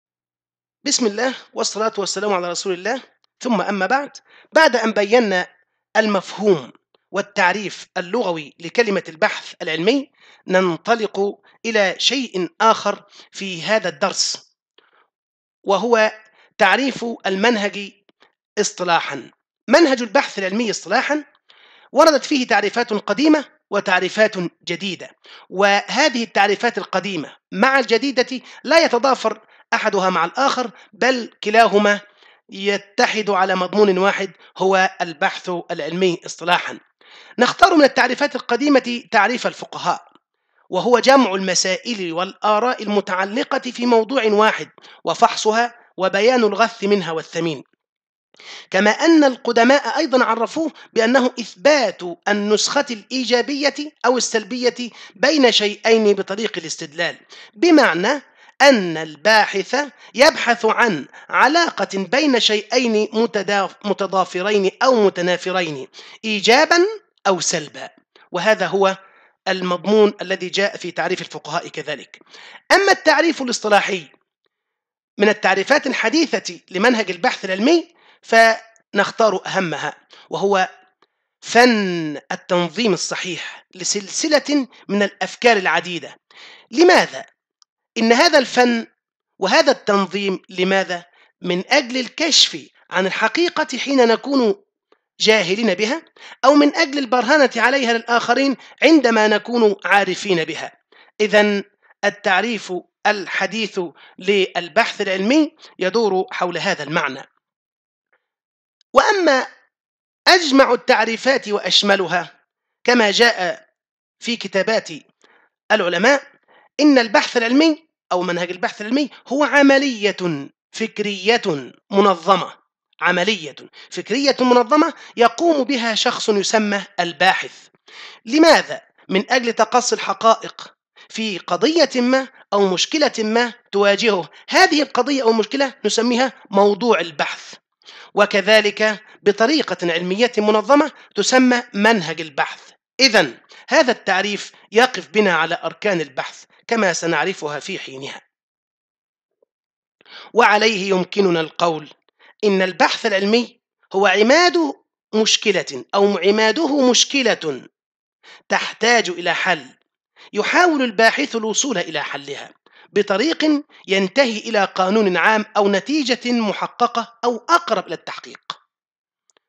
بحث ومصادر | الوحدة 1 | تعريف منهج البحث العلمي
المقررات المفتوحة - بحث ومصادر 291 عقد 2
الوحدة الأولى : البحث العلمي تعريفه – خصائصه - مجالاته
الدرس 2 : تعريف منهج البحث العلمي
تعريف الفقهاء
- التعريف الحديث لمنهج البحث
- عماد البحث.